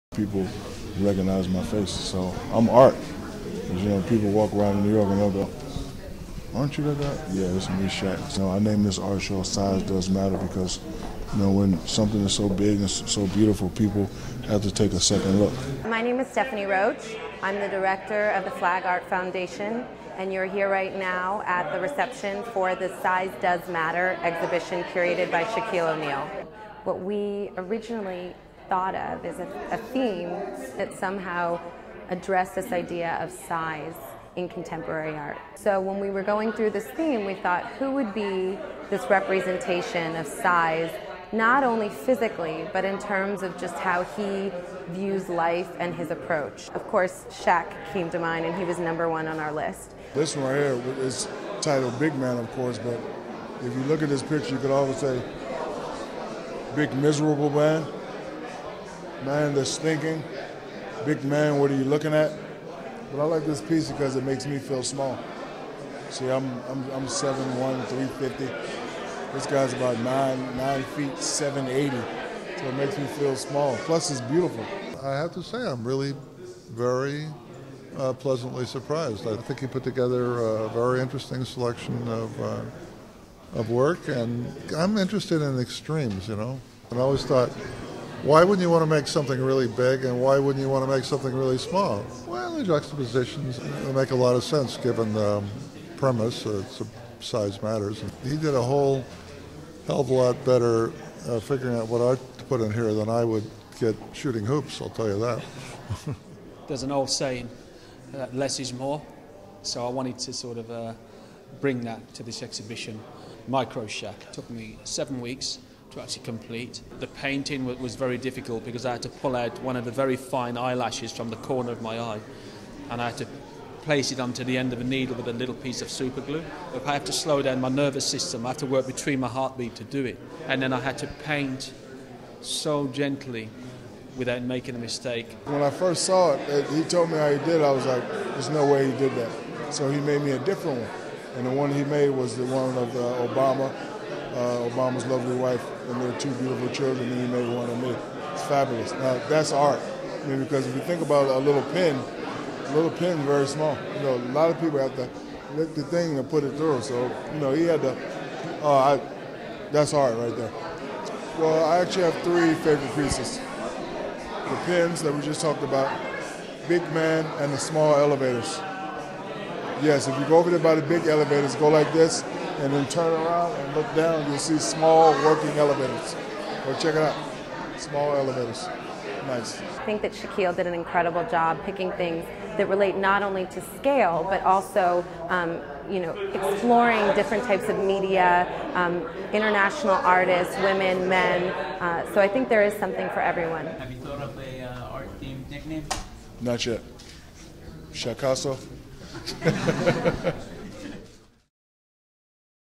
Shaq Attacks Art World in 'Size DOES Matter'
When the FLAG Art Foundation decided it wanted to showcase an exhibition of works exploring how scale is used in contemporary art, it decided to expand its search beyond the art world to find a curator.  The foundation thought "big." Immediately, one name came to mind: Shaq. That's NBA superstar Shaquille O'Neal.